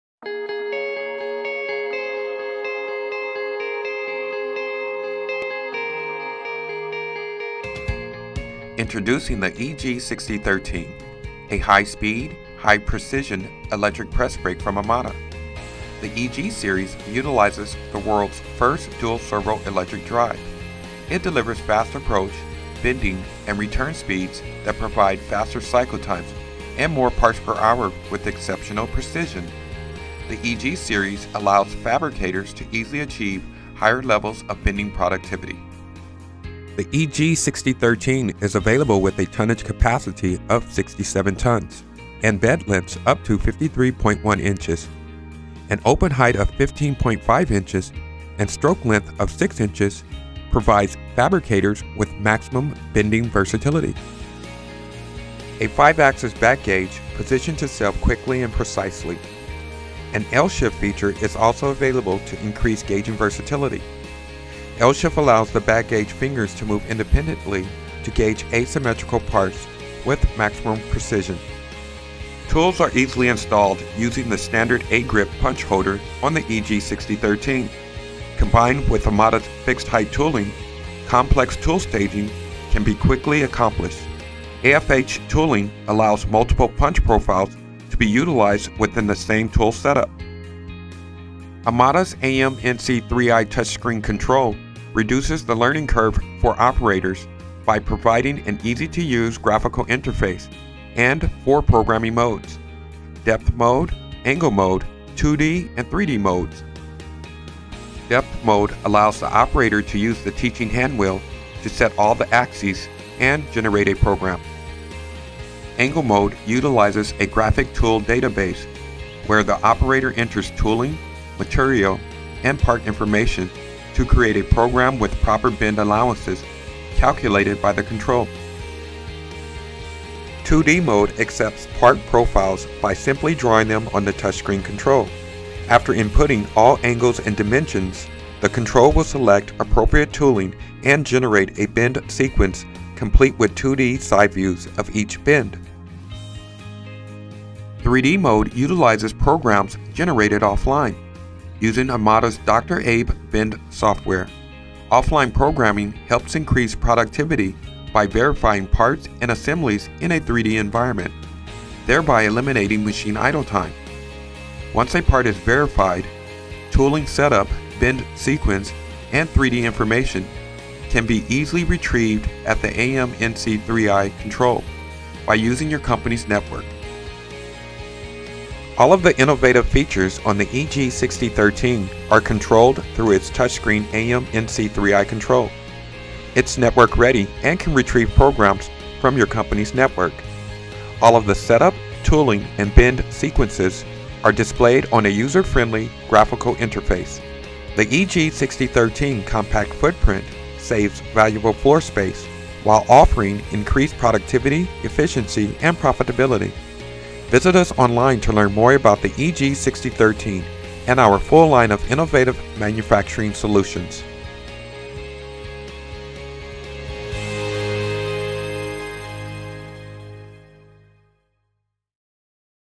EG 6013 Compact Press Brake
The EG 6013 is an ultra-high precision, high-speed compact bending solution featuring an advanced and precise Dual Servo Power drive system.

Features of the EG 6013 press brake include:
— Dual Servo Power (DSP) Drive - Extremely fast approach, bending and return speeds – provide faster cycle times and results in more parts per hour.
—AMNC 3i - An 18.5-inch multi-touch display offers multiple modes of data entry (angle, depth, 2D and 3D) for the utmost in programming flexibility.
— L-Shift Back-Gauge – 5-axis back gauge speeds setup for complex parts. High-speed movement on all axes ensures that the gauge fingers are positioned as quickly as the operator positions the part.